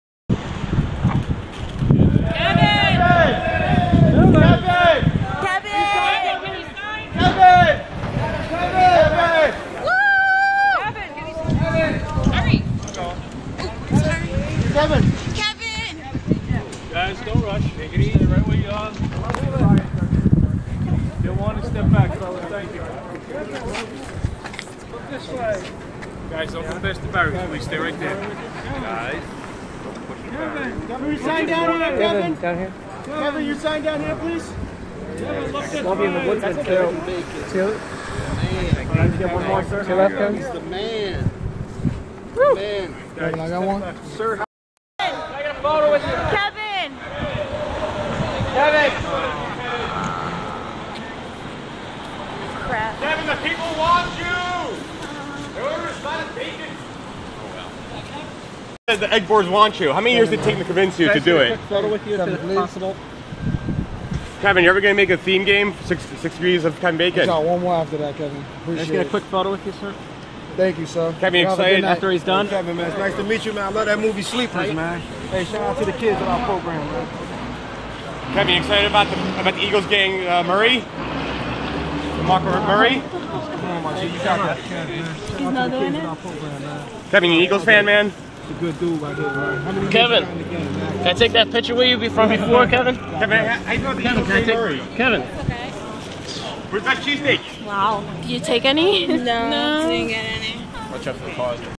Kevin Bacon at The Late Show 3/13/15
Kevin Bacon signed autographs for fans.